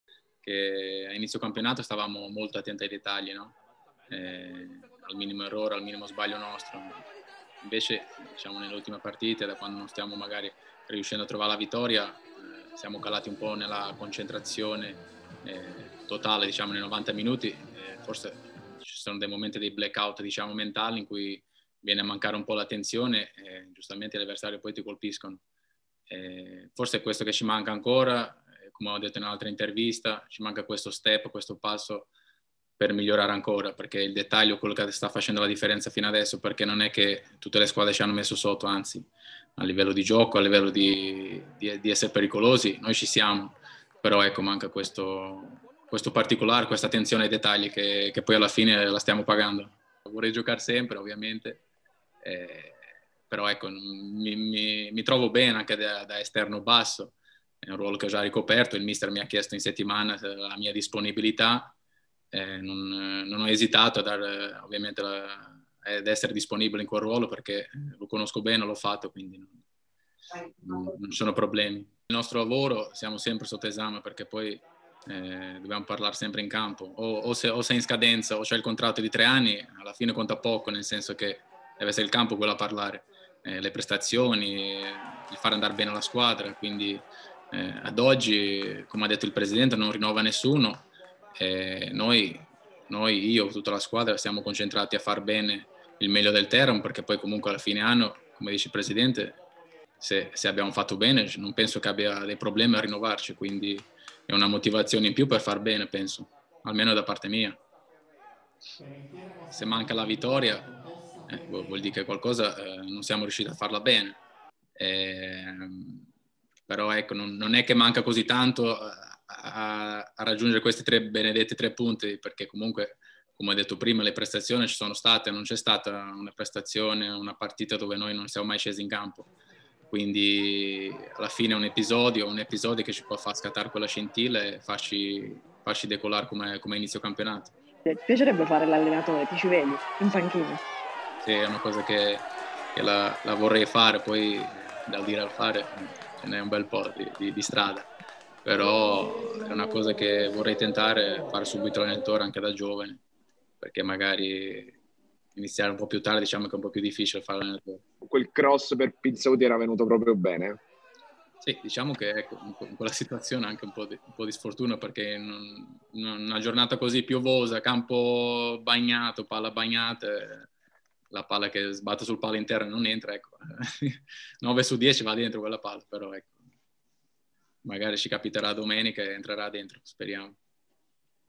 Costa Ferreira: “Da evitare i black-out mentali”
Il centrocampista del Teramo, Pedro Costa Ferreira torna sul pareggio di sabato contro l’Avellino. Gara nella quale ha interpretato il ruolo di terzino destro. Il portoghese ammette che vanno evitati cali di concentrazione e ha ammesso il desiderio di diventare allenatore una volta tolti definitivamente gli scarpini